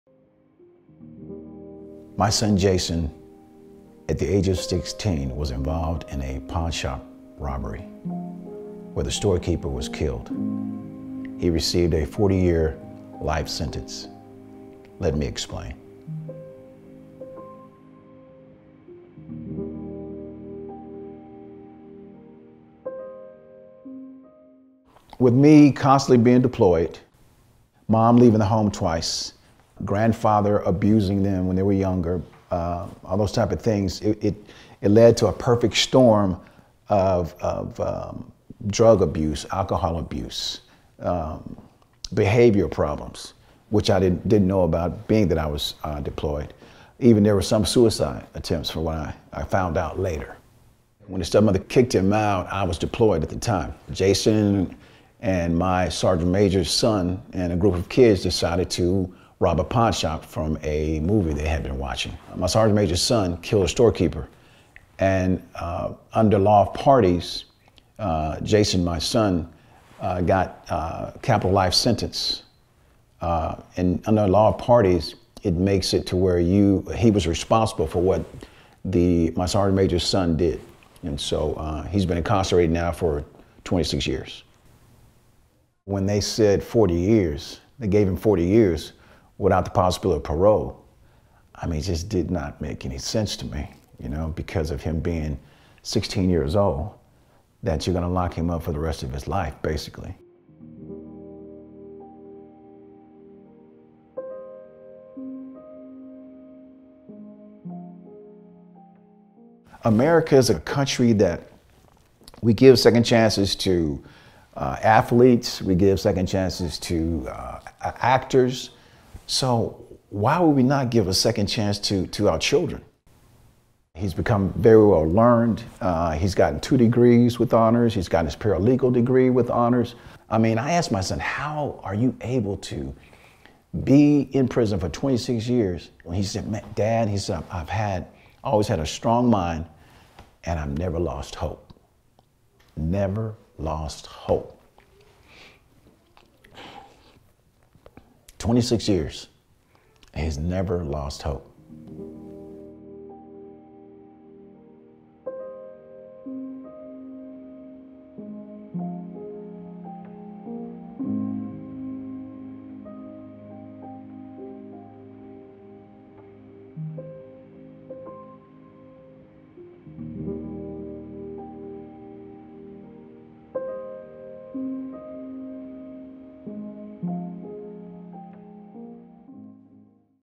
Let Me Explain: Larry's Story
When Larry was deployed in the military, his son took part in a robbery where a store clerk was killed. Although Jason did not pull the trigger, he was charged with capital murder under the law of parties. Larry’s son has spent 26 years behind bars on a 40-year sentence, and in that time he’s received multiple degrees and has “never lost hope.”

In Texas, kids can be locked up before their brains are even fully developed, losing the change to build a meaningful life outside of prison walls. Second Look legislation would give them an earlier opportunity to be considered for parole, cutting their mandatory sentences from 40 years to 20. Learn more about this issue on TCJC’s